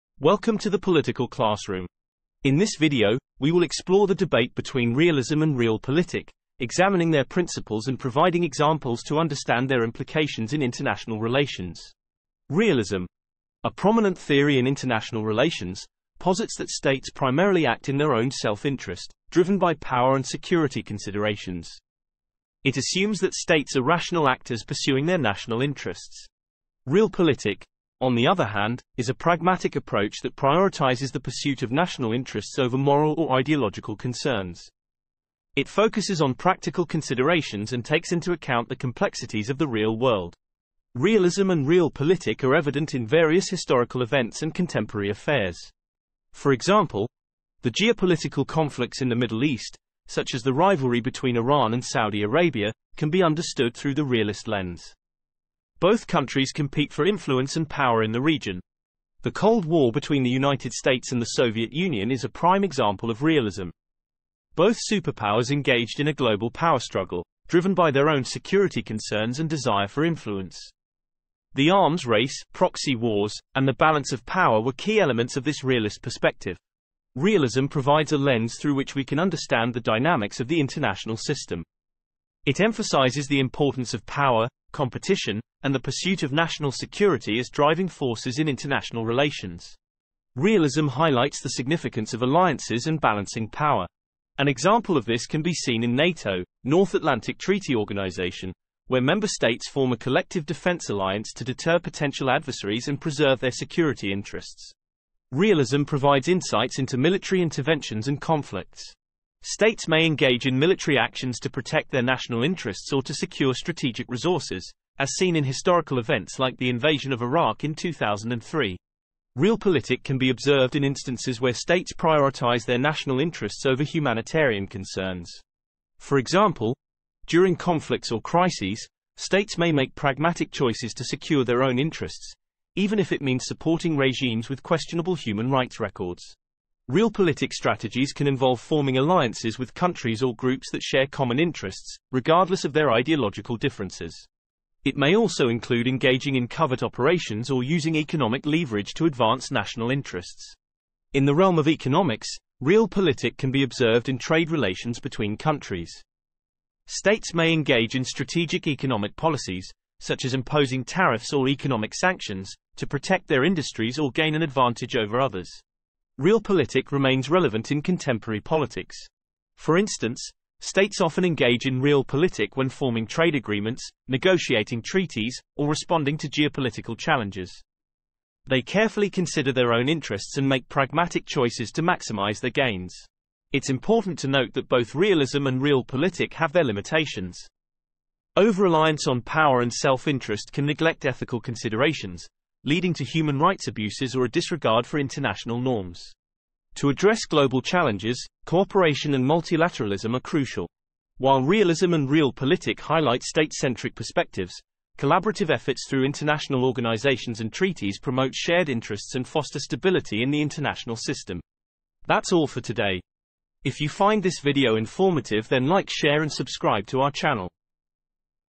The Power Play: Realism vs. Realpolitik in International Relations!
Welcome to our in-depth exploration of the contrasting concepts of realism and realpolitik in the field of international relations. In this thought-provoking video, we delve into the complexities of these perspectives, which shape our understanding of global politics. Join us as we analyze the key tenets of realism, emphasizing the importance of state interests, power dynamics, and the balance of power in international affairs. We also navigate the theory of realpolitik, which focuses on pragmatic decision-making, strategic calculations, and the pursuit of national objectives. Discover the nuances and debates surrounding these theories, and gain insights into their implications for foreign policy and international security. Don't miss this engaging discussion as we dive into the world of realism vs. realpolitik in international relations!